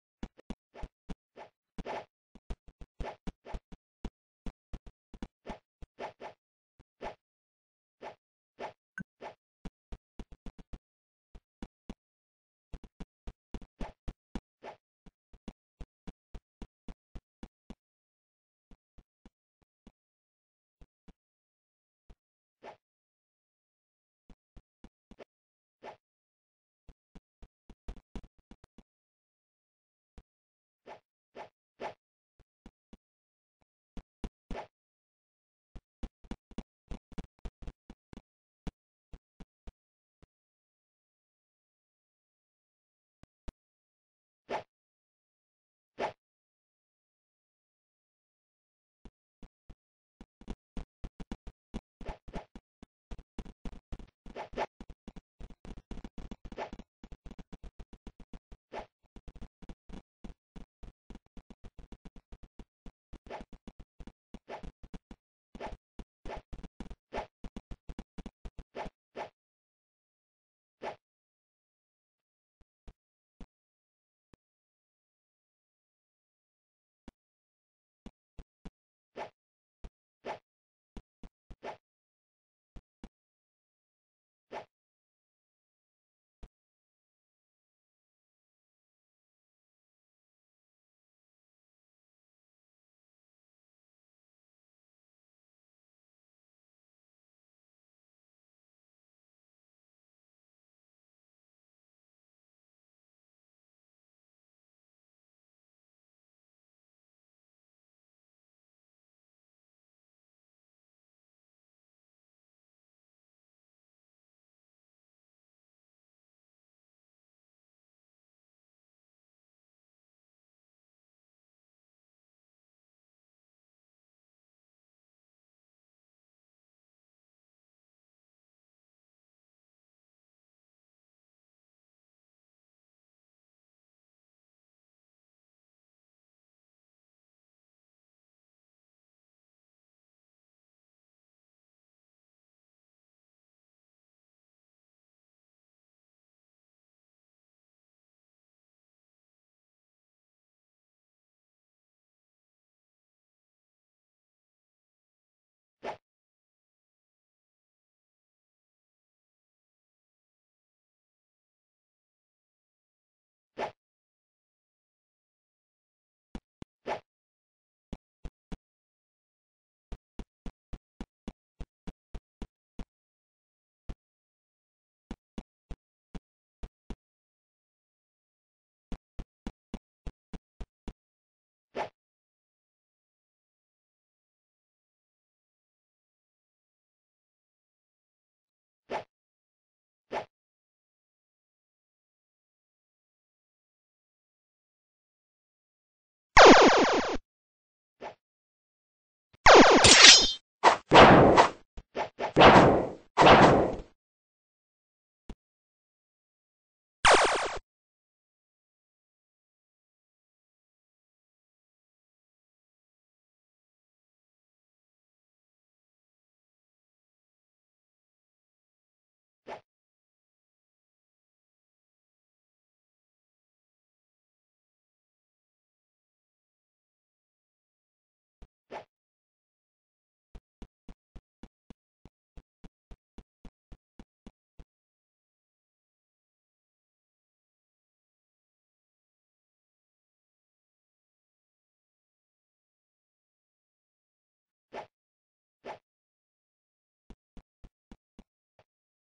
Little bro wrecks wild meeting.
We will hunt down d4 leader wild and get his blood.coward. I was going to kill him with my account but he shutdown game when i came.